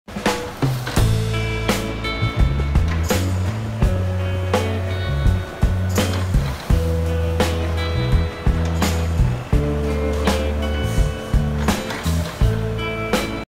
Trim paper
Our JCB grab handling trim paper 📝 before baling! ♻️